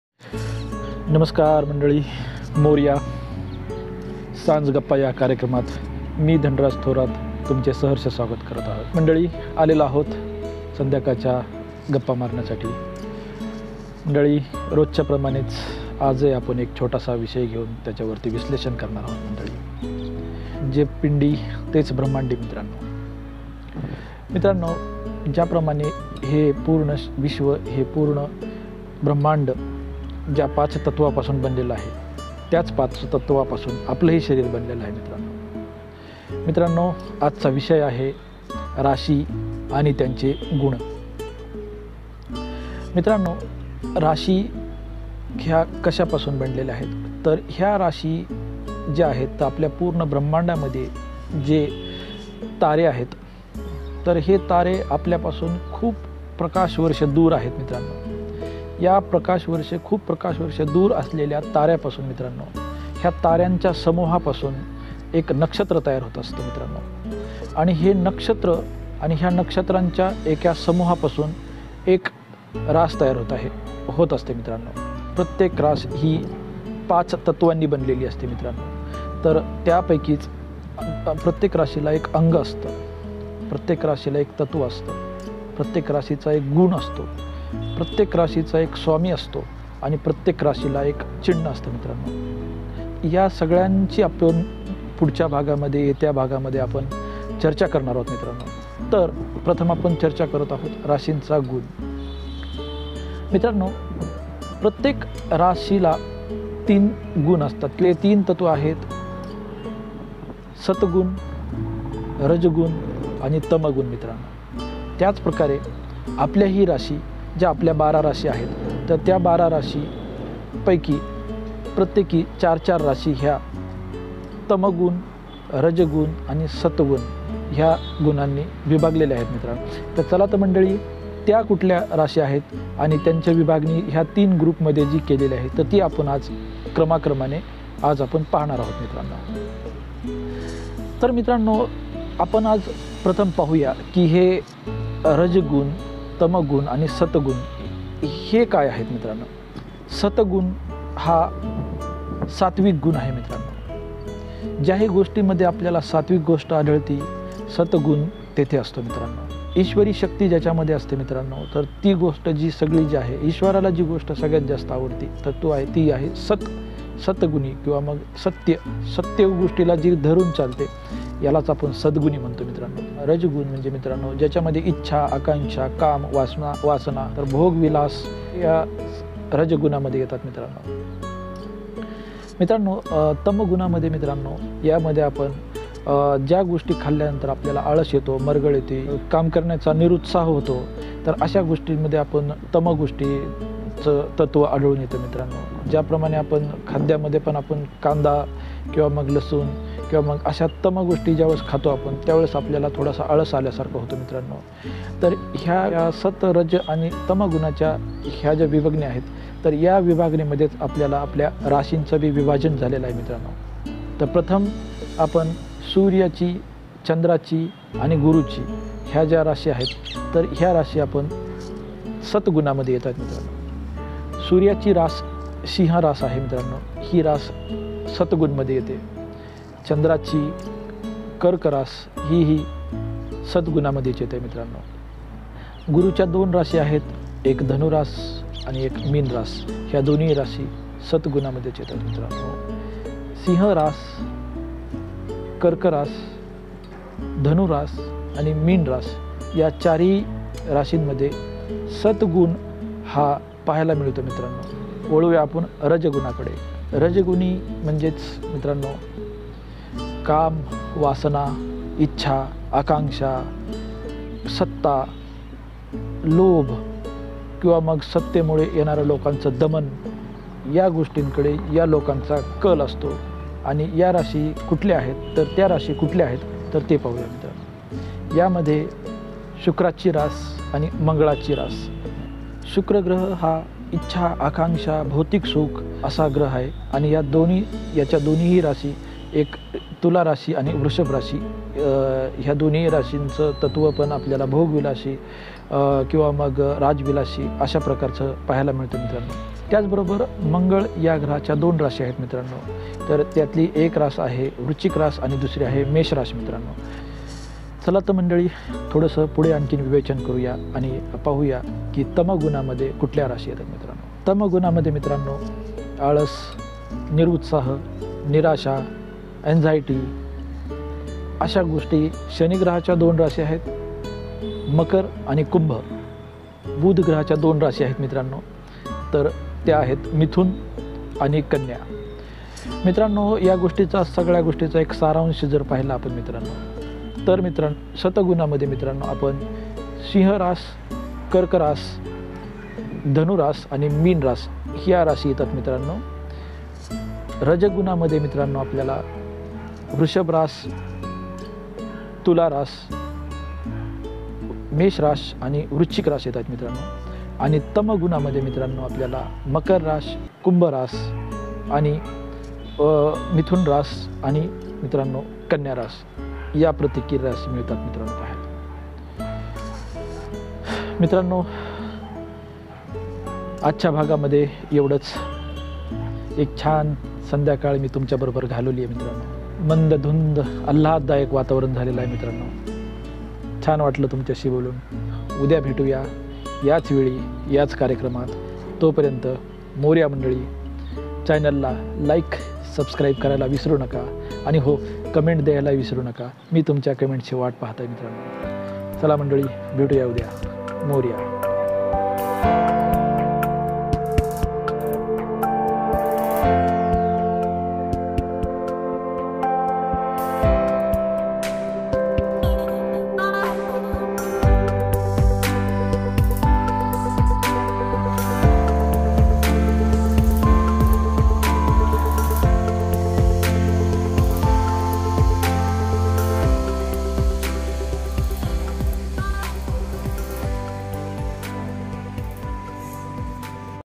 राशींचे गुण | सांज गप्पा
Program :- सांज गप्पा
Topic :- राशींचे गुण
Day :- 4
By :- Dhanraj Thorat